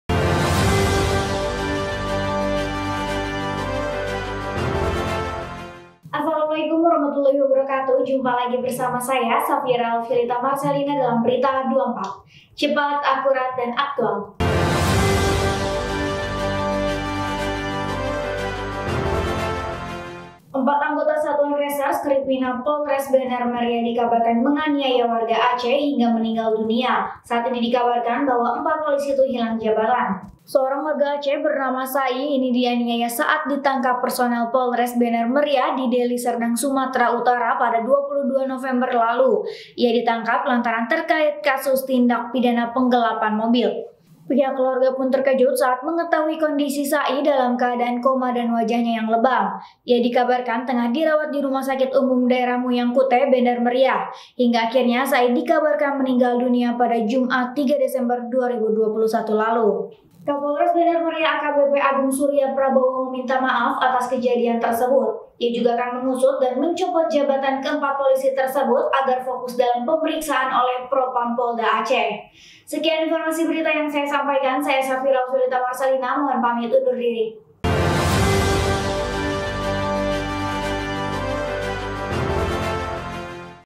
Berita24.Com - Dugaan Aniaya Bikin Warga Aceh Tewas Berujung 4 Polisi Hilang Jabatan
Empat anggota Satuan Reserse Kriminal Polres Bener Meriah dikabarkan menganiaya warga Aceh, hingga meninggal dunia. Saat ini dikabarkan bahwa, empat polisi itu hilang jabatan.

- Every Content in this Channel for the purpose such as Education, News Report, interpretation etc.